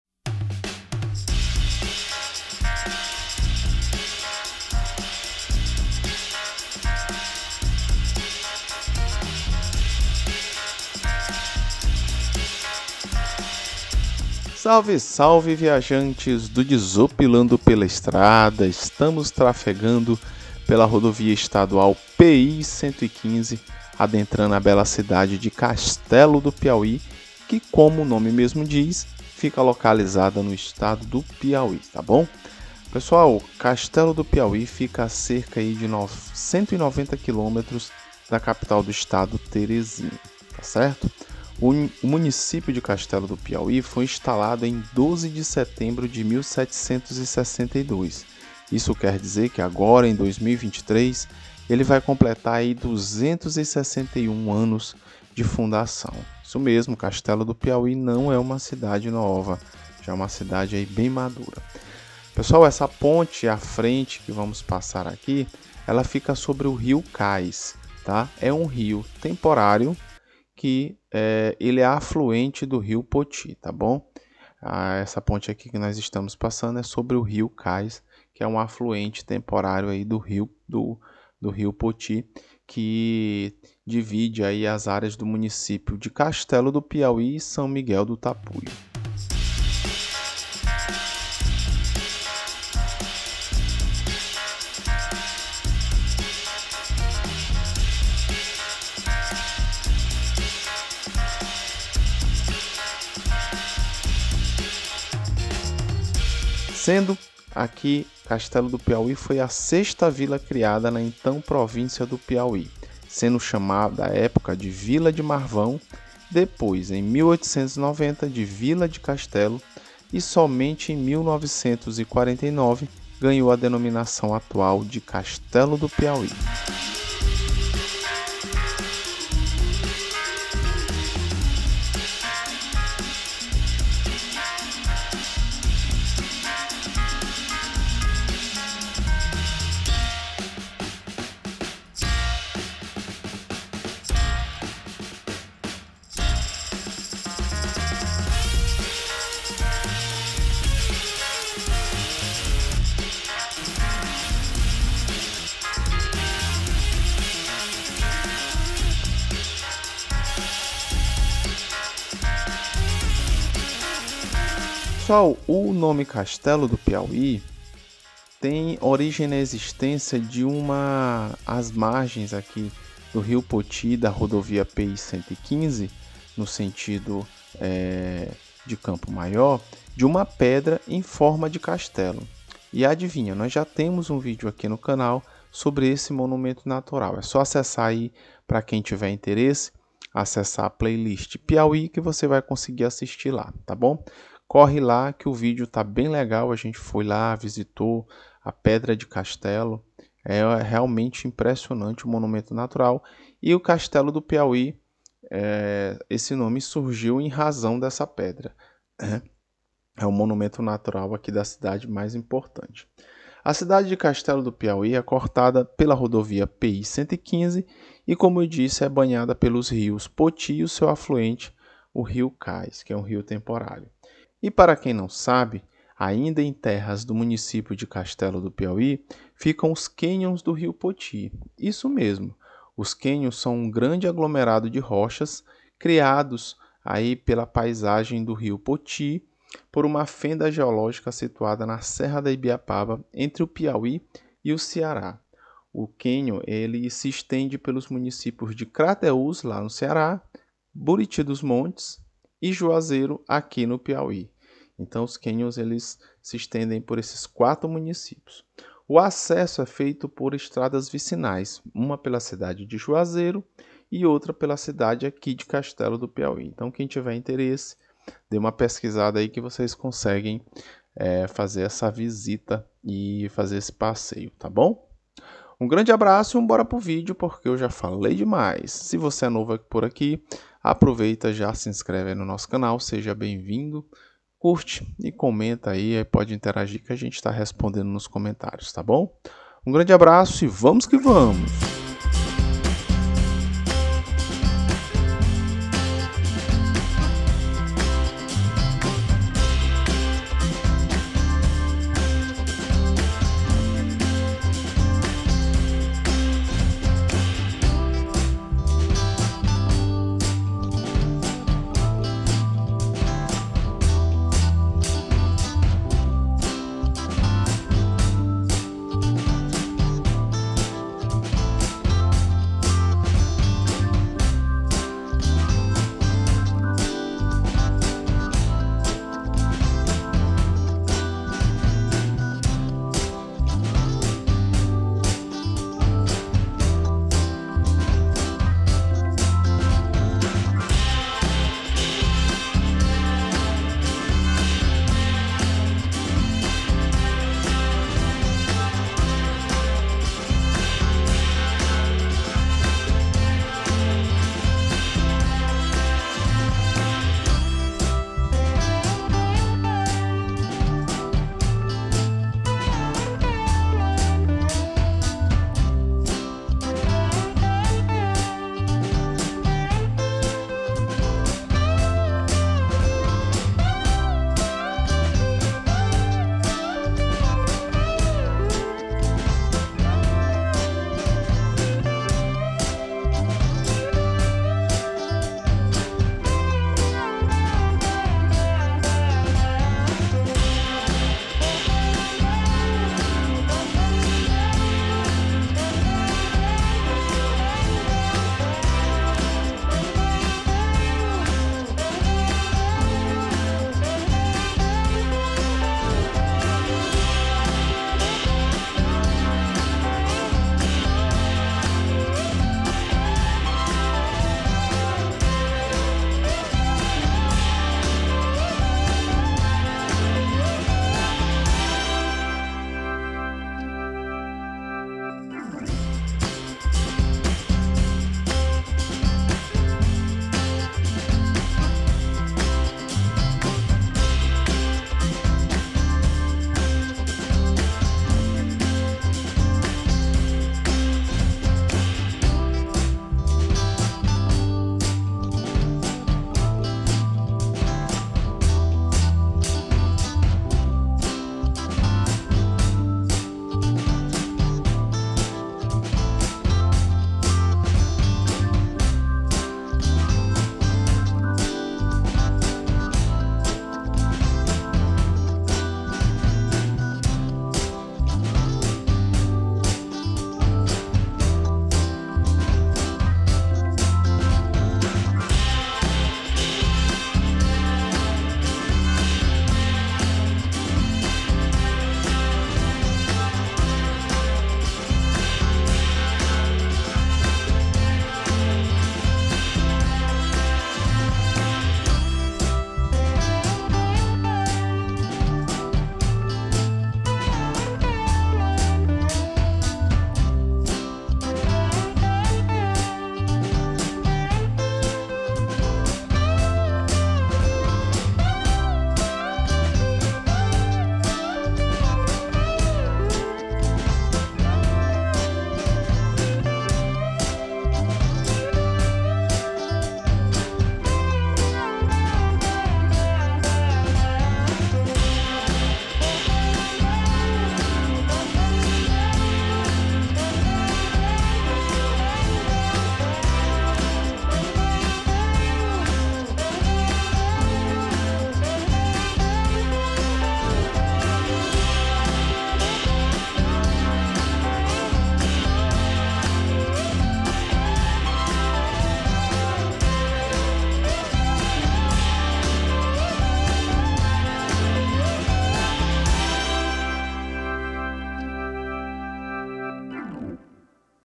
Castelo do Piauí, a cidade com o portal mais bonito!
Salve Viajantes!

Tudo bom?!

Hoje vamos dar uma volta rápida pelo município de Castelo do Piauí!

Se liga e vem curtir com a gente!

Forte abraço,

Márcio Rodrigo